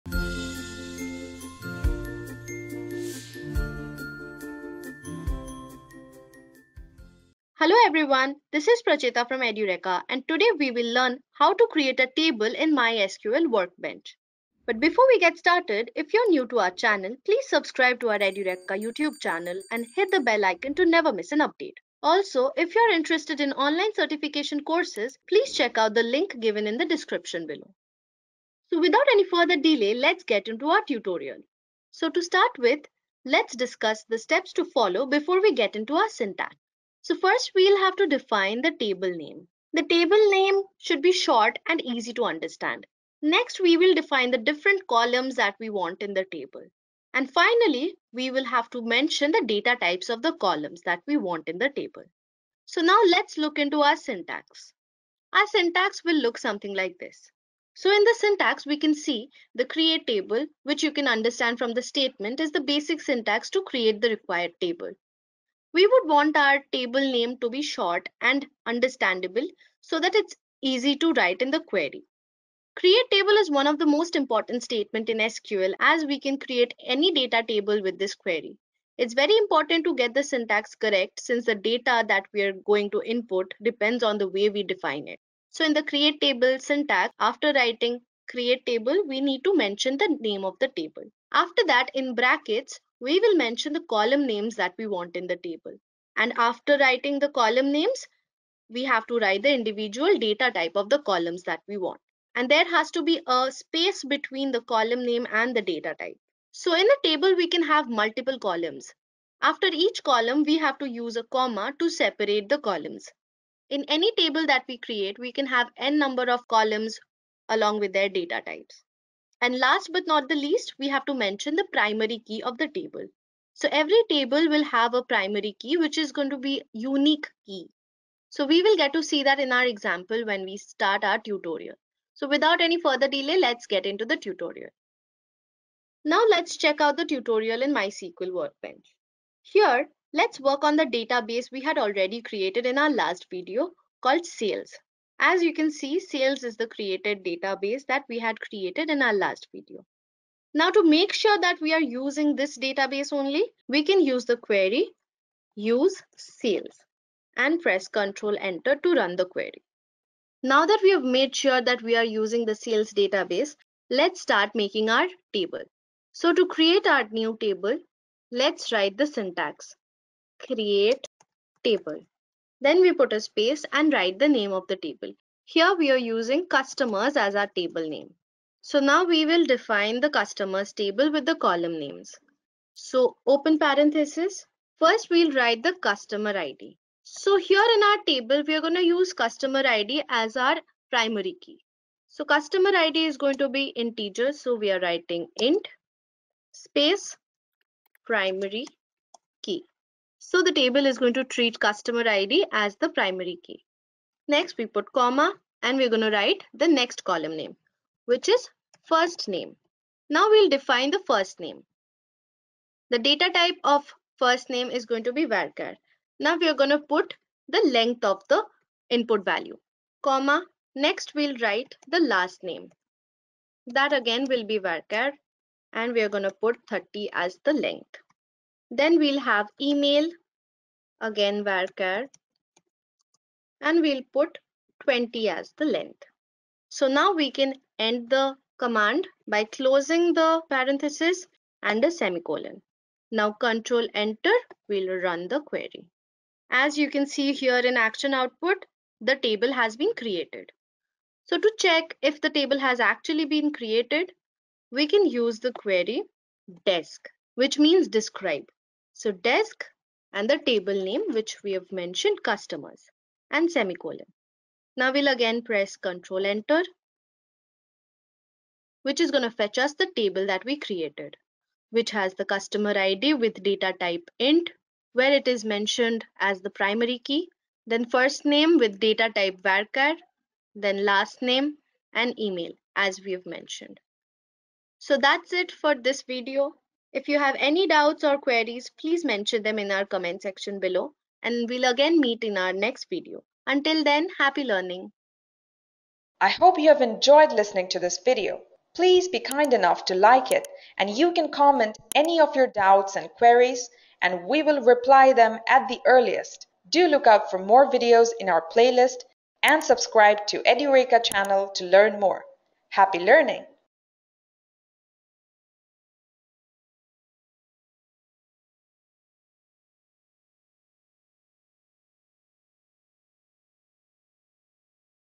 How to Create Table in MySQL | How to Create Table in MySQL Workbench | Edureka
This Edureka video on "How to Create Table in MySQL" will introduce you to the tutorial of how to create a table in MySQL Workbench. We will discuss the syntax and understand the hands-on of how to start creating a table. Let us know if you have any more points to add in the comments below.

Following pointers are covered in this How To Create a Table in SQL video:
00:00 Introduction
00:36 Steps to Follow
00:54 Syntax
02:16 How to Create a Table in SQL Tutorial

𝐄𝐝𝐮𝐫𝐞𝐤𝐚 𝐏𝗼𝘀𝘁 𝗚𝗿𝗮𝗱𝘂𝗮𝘁𝗲 𝐂𝐨𝐮𝐫𝐬𝐞𝐬
🔵 Artificial and Machine Learning PGD with E & ICT Academy NIT Warangal